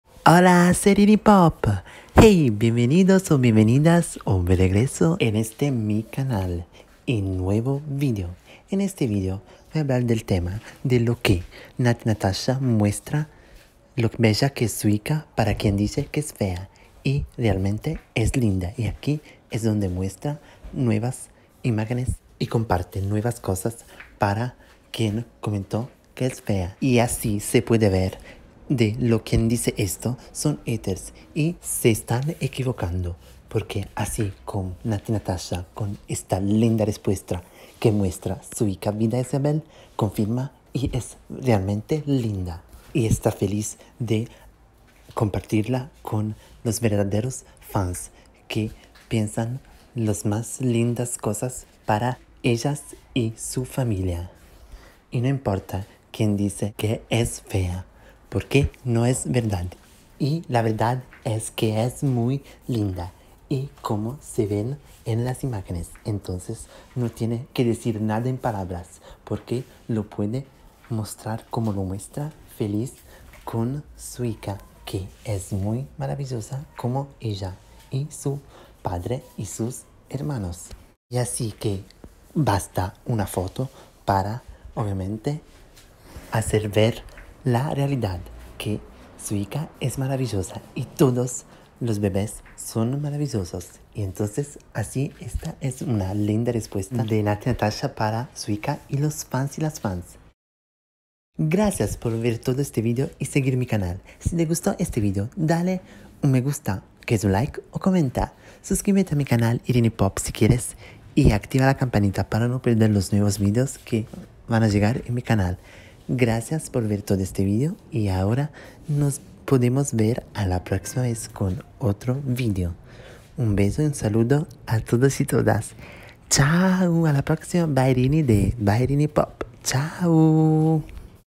Natti Natasha Muestra A Quién Dice Que Su Hija Es Fea Lo Bella Que Es Su Hija Vida Isabelle
Hola, bienvenidos o buen regreso en mi canal, en este video te hablo del lindo modo de algunos comentarios y de cómo Natti muestra feliz su hija, buena visión.